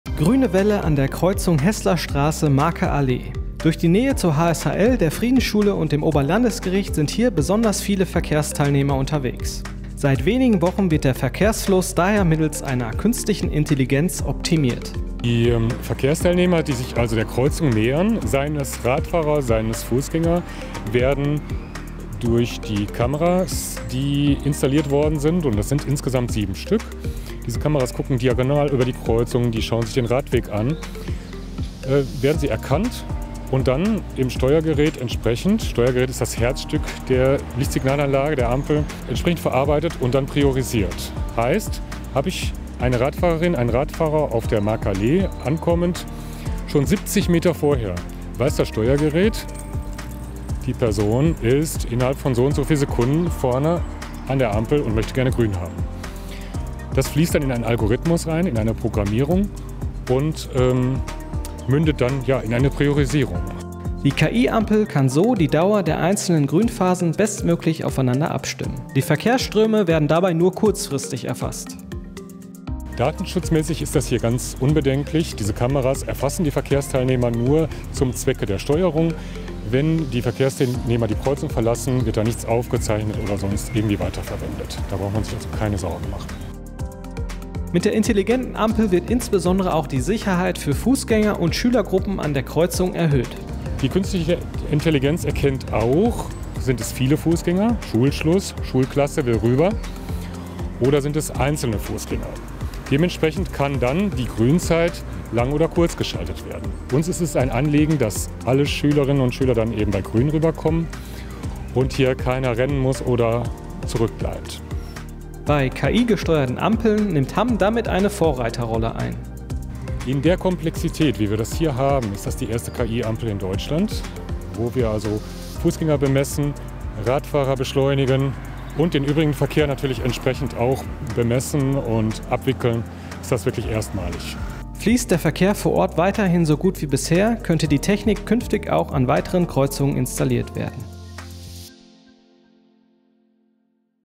Grüne Welle dank künstlicher Intelligenz in Ampelanlage in Hamm
💻📲 ChatGPT, Photoshop AI und Co.: Künstliche Intelligenzen entwickeln sich aktuell rasant fort. Auch im Straßenverkehr kommen sie zum Einsatz. So zum Beispiel bei einer KI-Ampel🚦, die deutschlandweit erstmalig bei uns in Hamm zu finden ist.